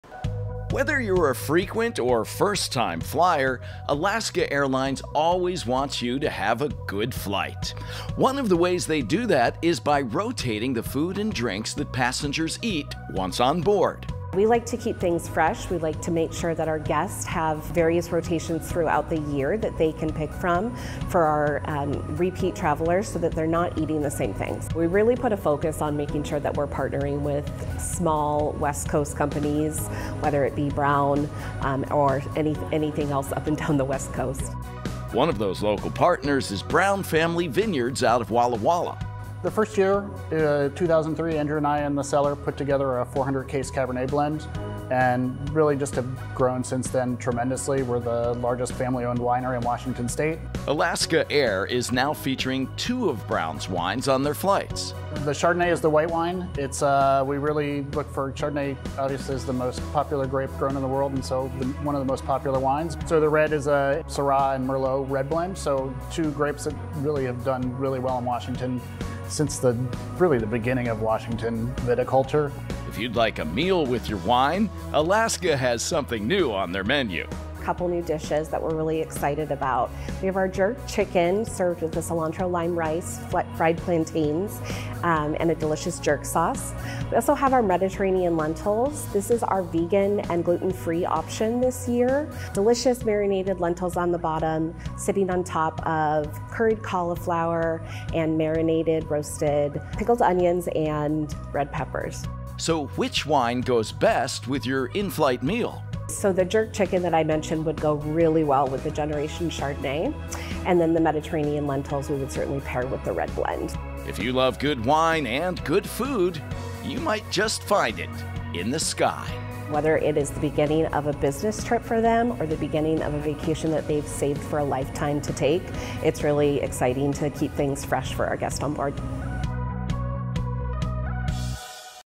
Alaska Airlines new food and wine menu keeps things fresh for frequent flyers
Did you know your taste buds dull at 35,000 feet?